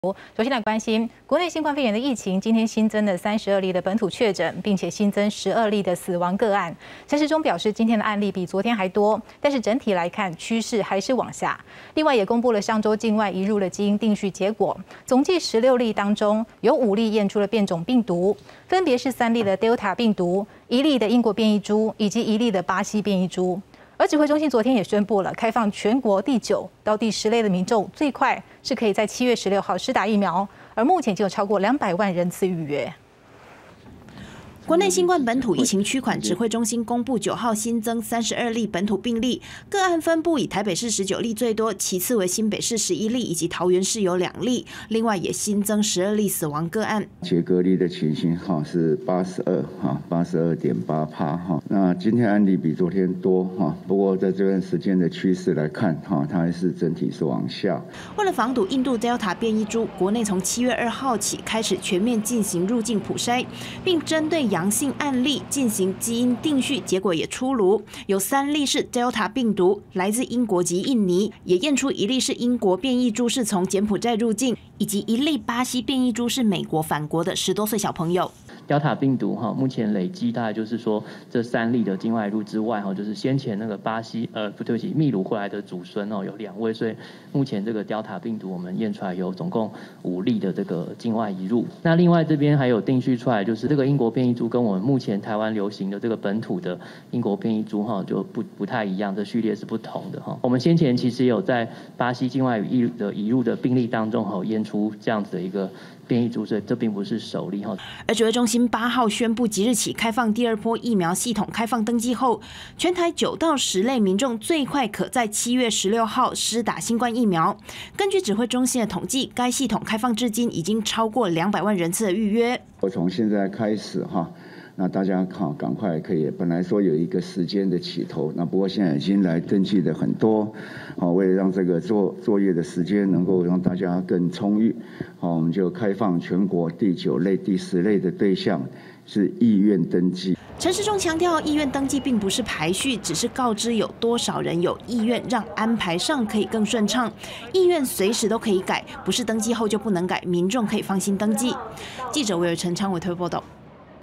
上週境外移入病毒基因定序 驗出包括印度、英國、巴西等變異株｜20210709 公視晚間新聞
國內新冠肺炎疫情，今天新增32例本土確診，並新增12例死亡個案，陳時中表示，今天案例比昨天多，但整體來看，趨勢還是往下，另外也公布上週境外移入基因定序結果，總計16例當中，有5例驗出變種病毒，分別是3例Delta病毒，1例英國變異株，以及1例巴西變異株。指揮中心昨天宣布，開放全國第9至第10類民眾，最快可在7月16日施打疫苗，目前已經超過200萬人次預約。